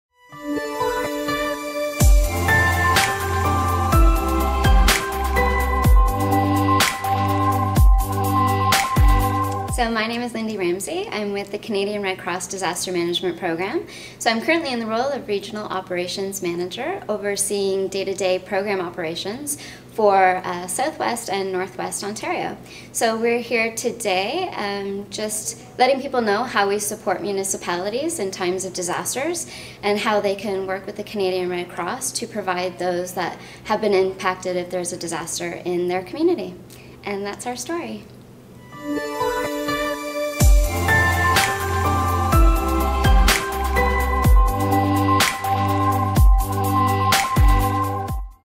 Supporting municipalities in times of disaster
At the 2018 AMO conference in Ottawa, Ontario, Lyndie Ramsey from the Canadian Red Cross Disaster Management program explains how municipalities can work with the program to support their communities in times of disaster.